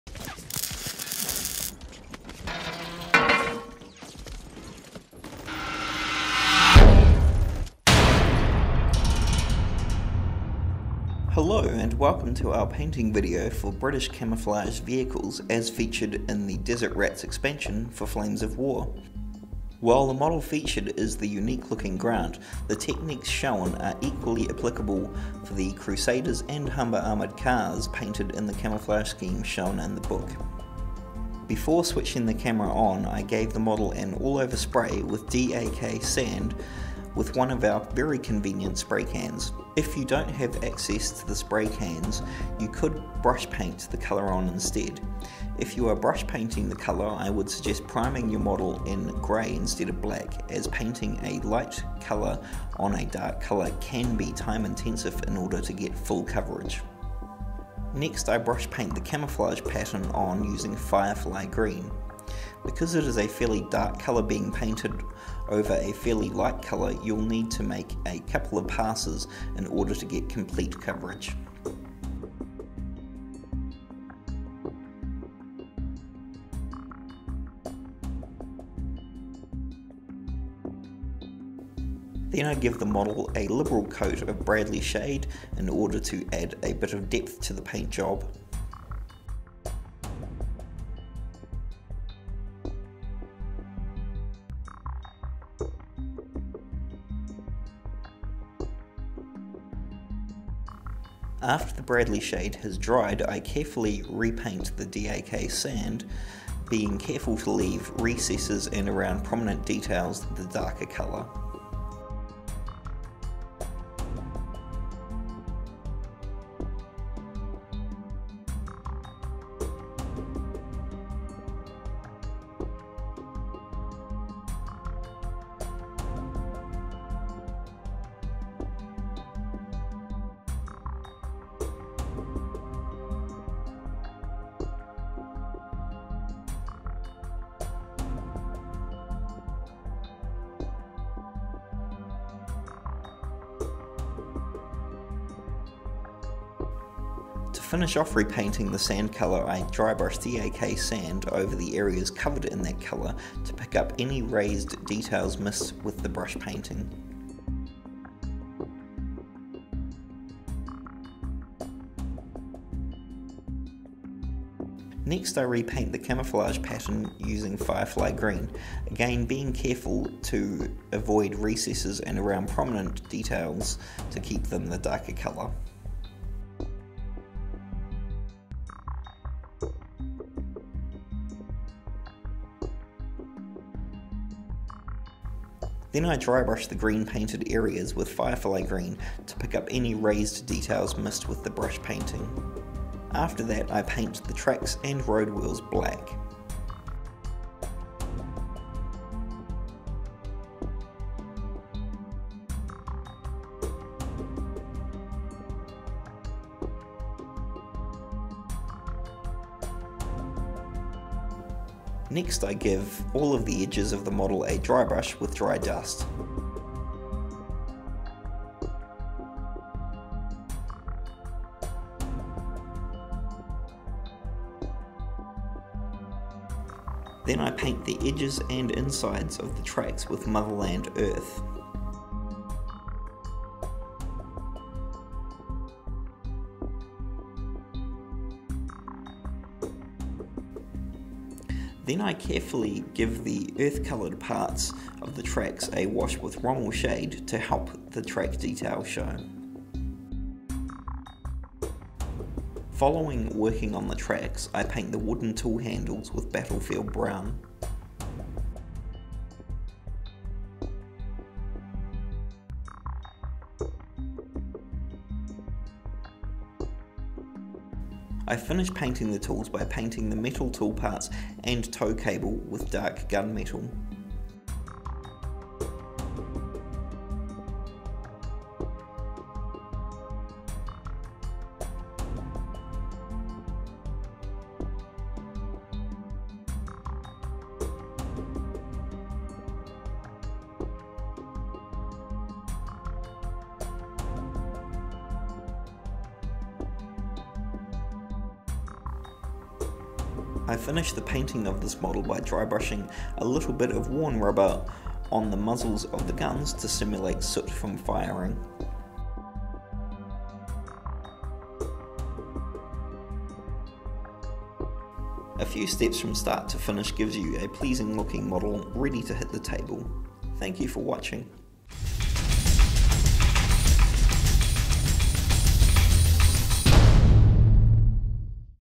Painting Guide - British Desert Armour
Aaron shows you how you can quickly and simply get your British tanks painted and on the tabletop.

to find out more.